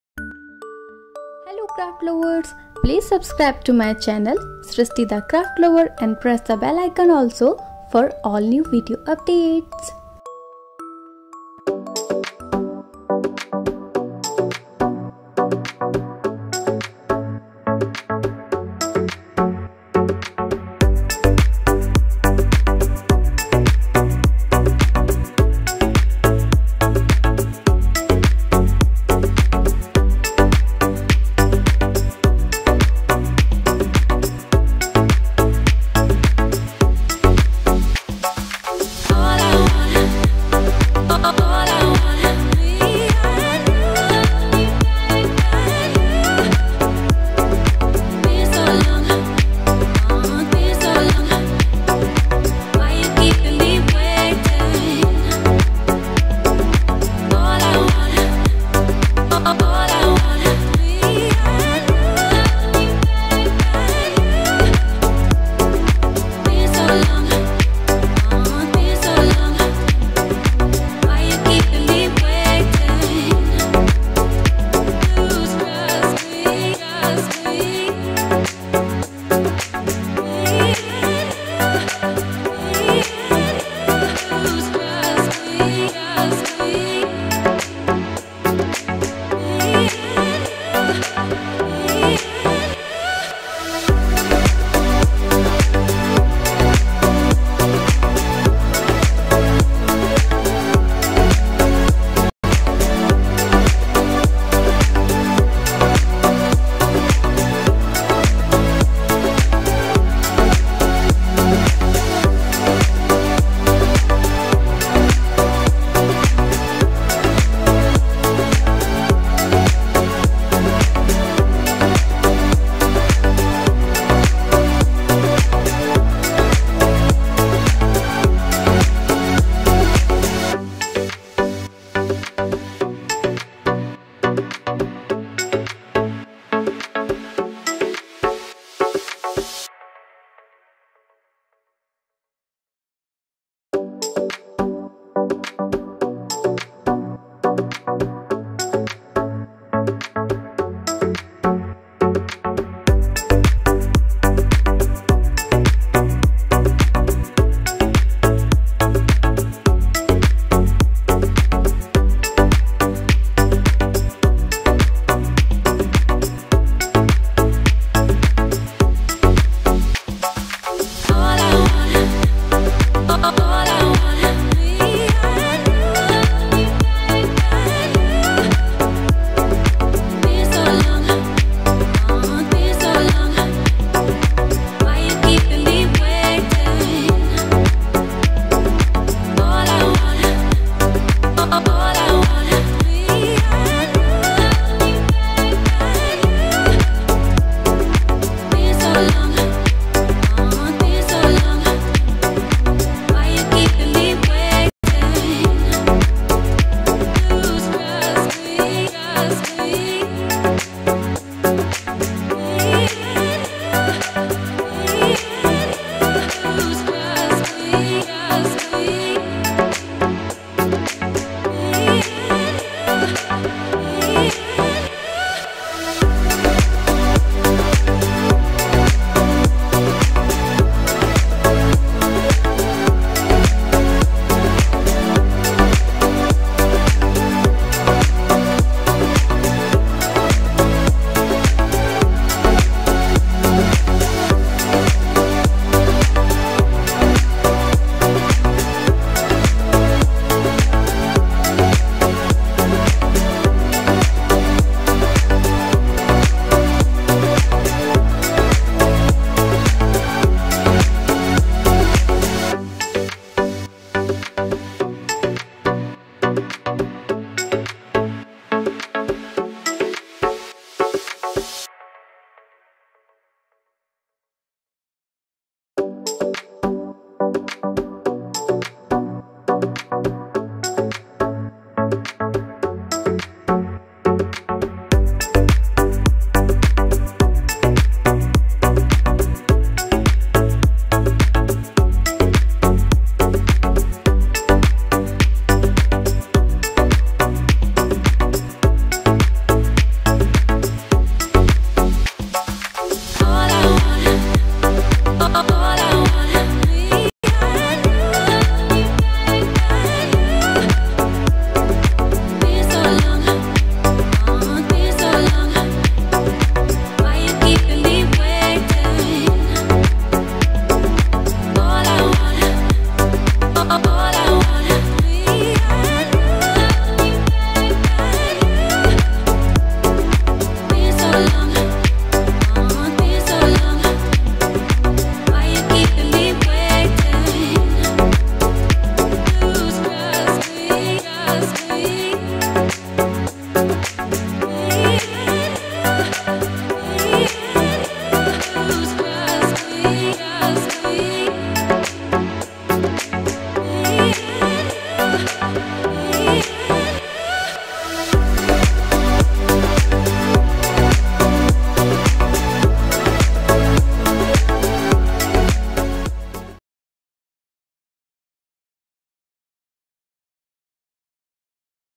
🥰 Surprise 🥰 Teacher's Day Handmade Card • teachers day card for sir & madam • card for teachers day
🙏🤩 Material required 🤩🙏

Light pink colour paper
white paper
beads moti white
colour paper extra
double sided tape
fevistick FEVICOL
scissor
sketch pen